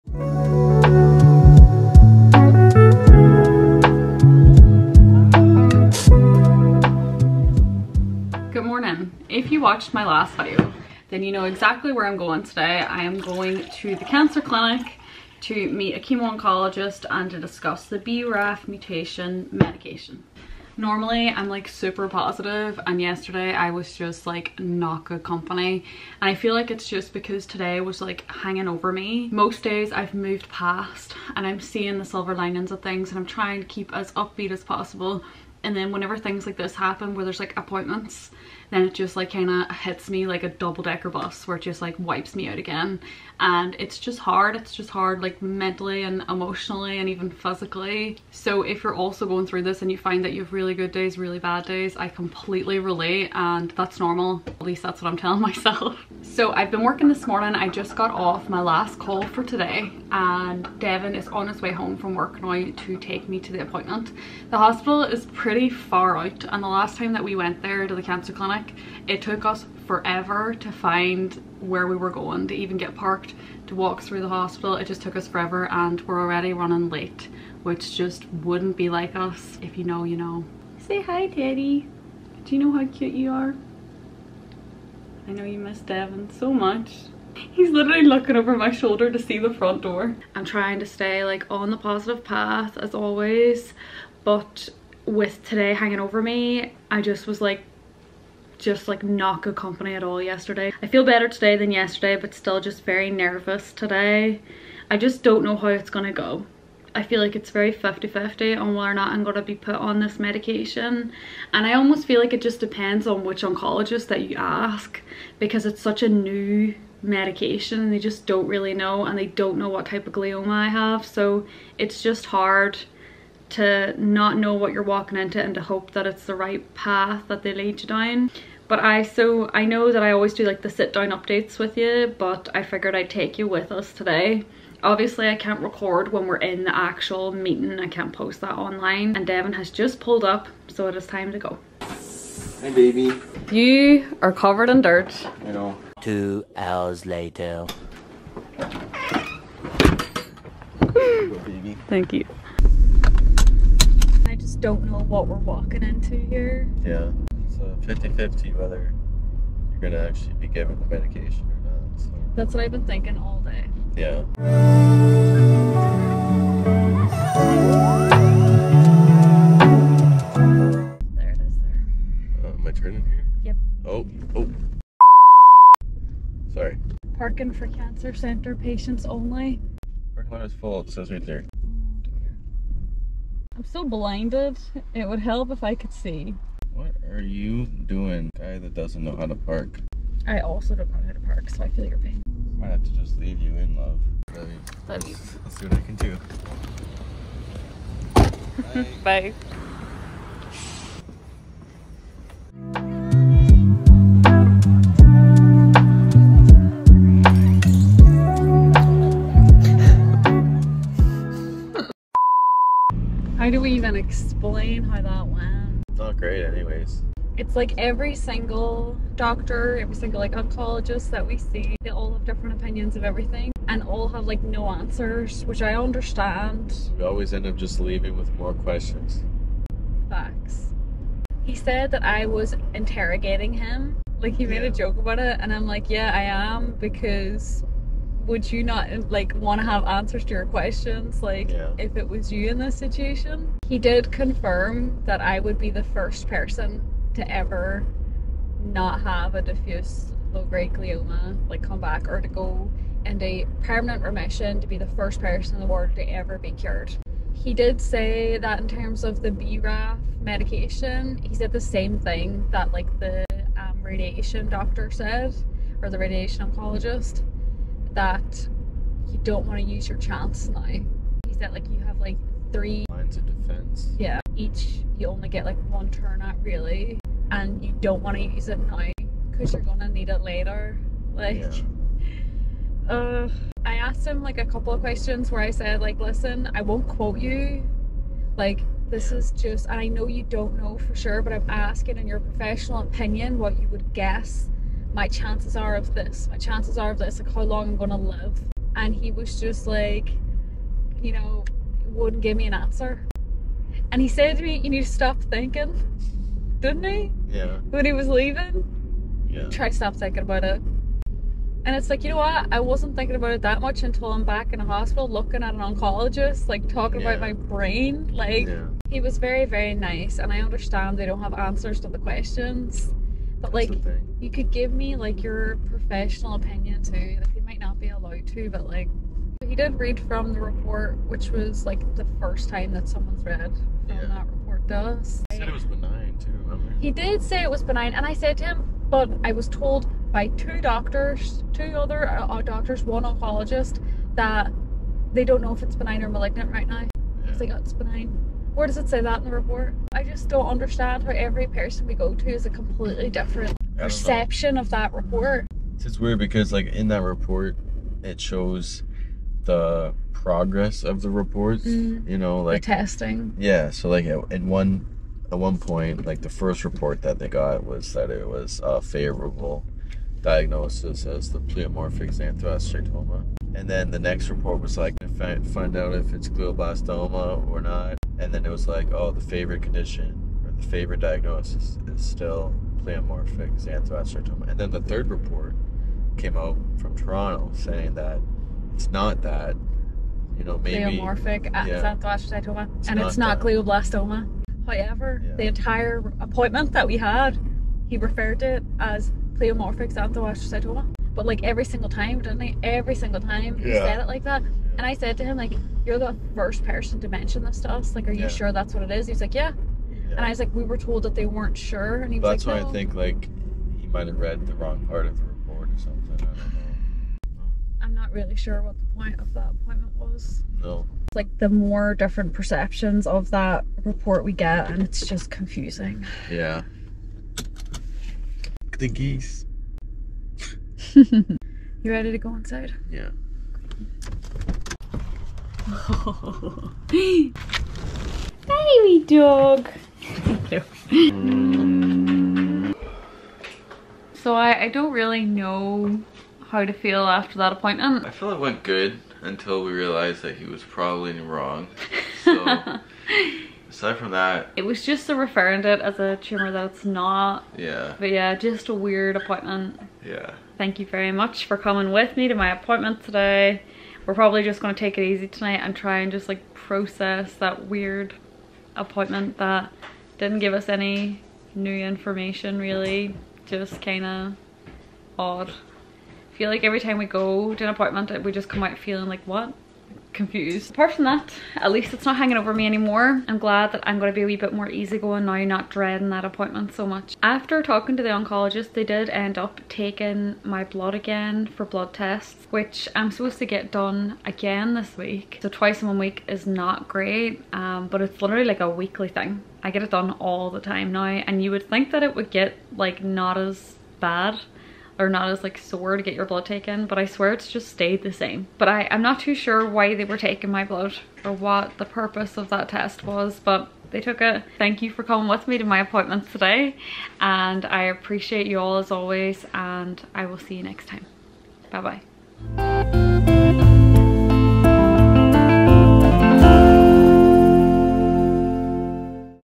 come to the cancer clinic with us 🩺 | oncologist appointment for BRAF V600E inhibitors vlog
I had an appointment with a chemo oncologist at the cancer clinic today to discuss the BRAFV600E inhibitor medication. I thought I'd vlog today instead of a sit down update video 😊 A very strange appointment... but we're glad we won't have it again any time soon!

My brain tumour was a grade II diffuse glioma with BRAF mutation - this is a specific as they can be with me unfortunately . It's not the xanthoastrocytoma that the doctor referred to it as throughout the appointment.

I will be sharing my brain tumour Facebook group soon 😊💗